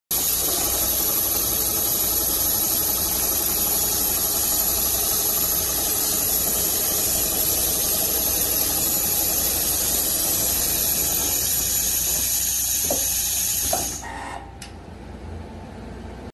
Richpeace leather sewing mahcine is testing on American customer's factory
Contact with Mr.Wade to get more details.